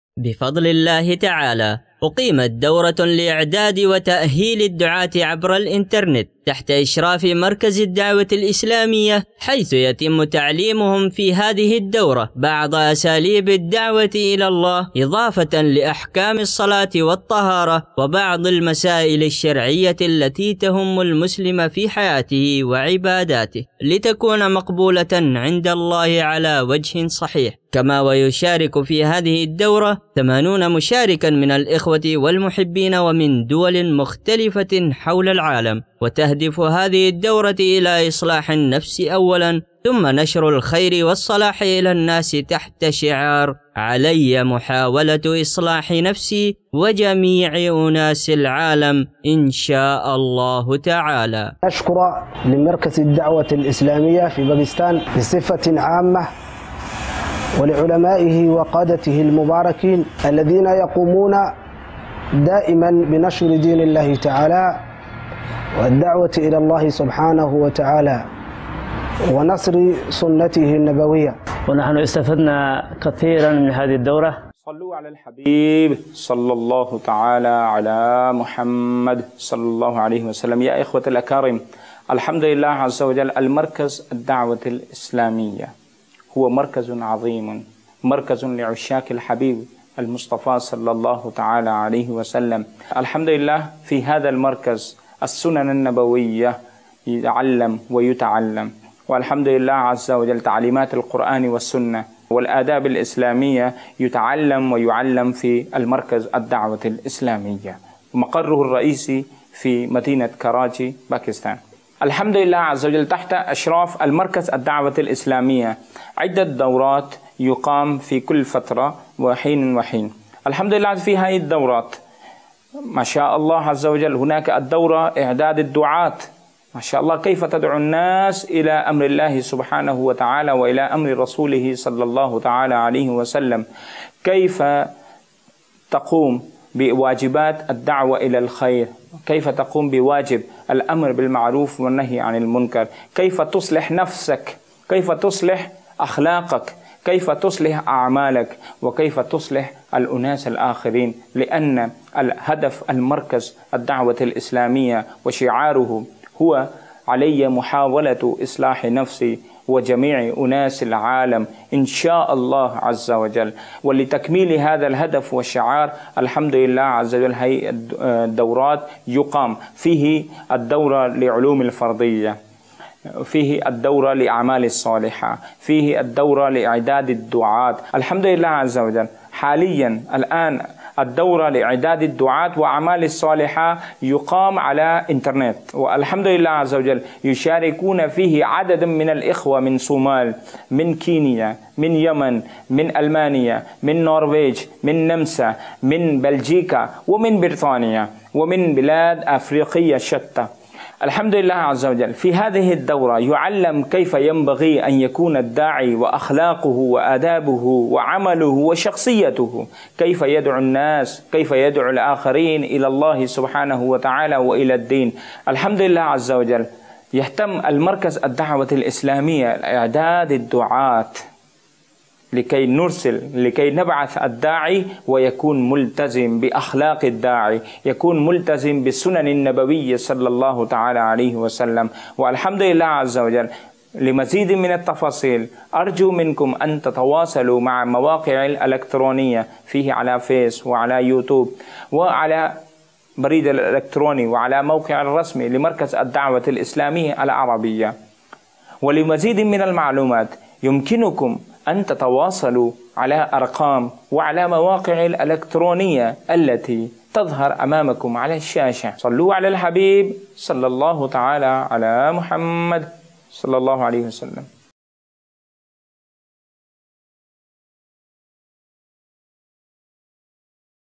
التقرير الأخباري || دورة إعداد وتأهيل الدعاة (عن بعد)
يحرص مركز الدعوة الإسلامية على نشر تعاليم الإسلام الحميدة كما يحرص على توجيه الدعاة وتدريبهم من خلال إلقاء الدورات والمحاضرات `وفي هذا الفيديو نقدم إليكم تقرير هاماً عن أحدى هذه الدورات وتوضيح أهدافها
ولمعرفة المزيد شاهد الفيديو

The Dawat-e-Islami is keen to spread the virtuous teachings of Islam and is keen to guide and train preachers by giving courses and lectures.

●▬▬▬▬▬▬▬▬▬ஜ۩۞۩ஜ▬▬▬▬▬▬▬▬▬●